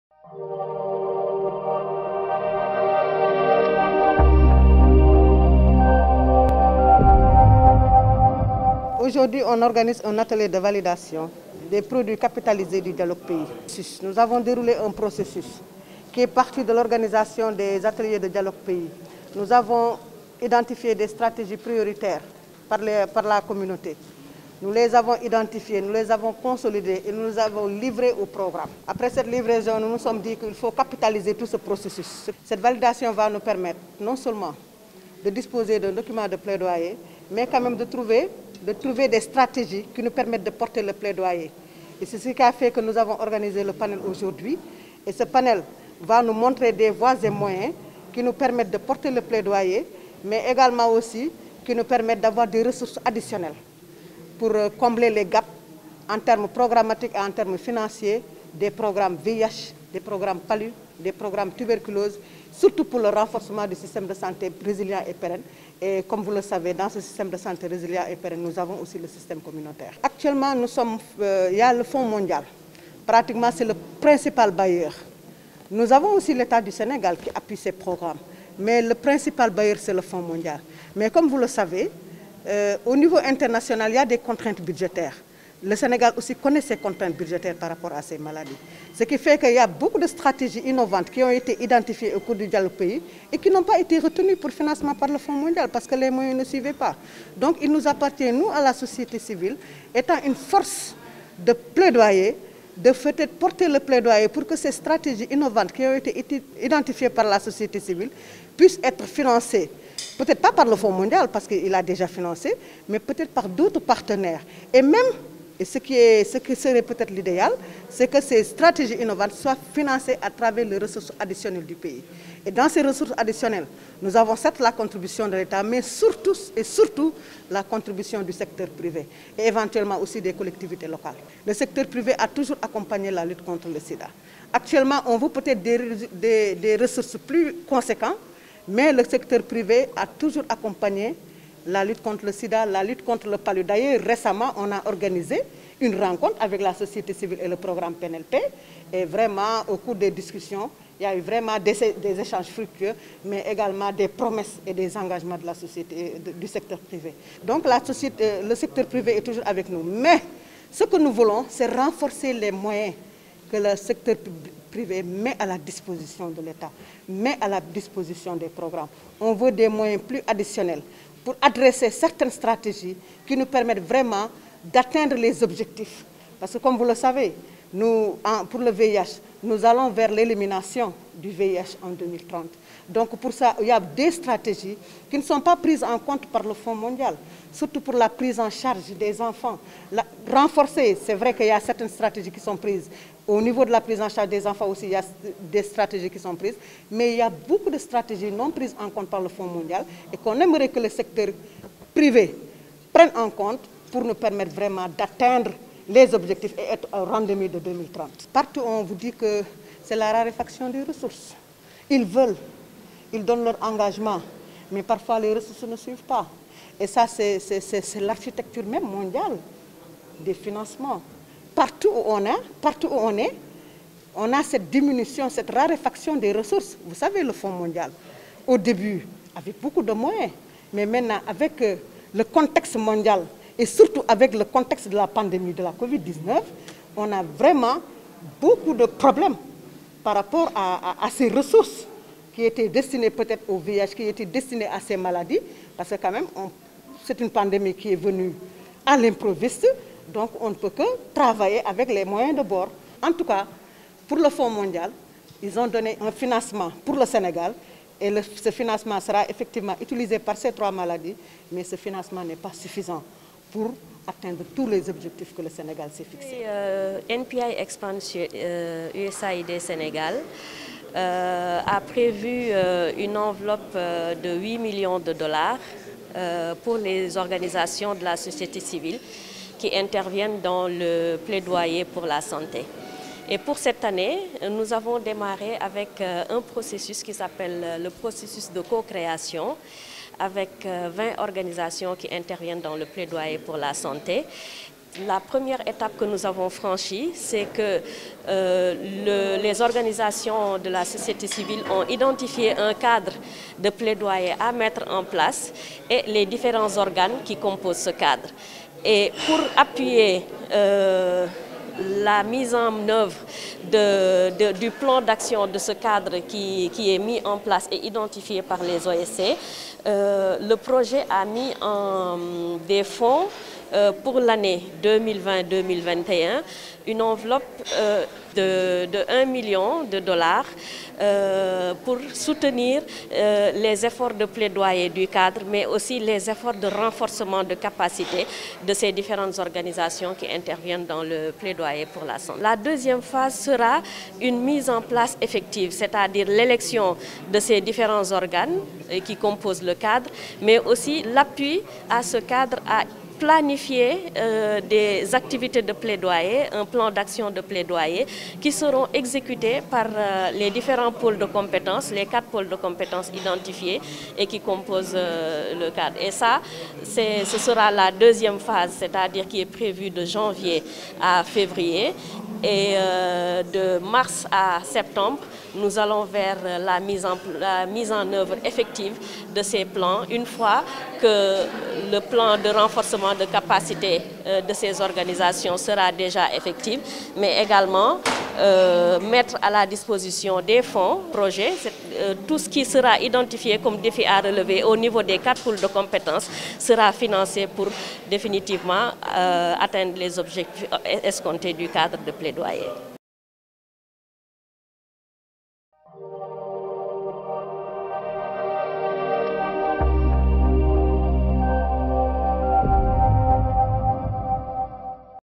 Sida, paludisme et tuberculose : Des Osc plaident pour leur prise en compte par les NMF3
Atelier de validation du processus de capitalisation :Sida, paludisme et tuberculose : Des Osc plaident pour leur prise en compte par les NMF3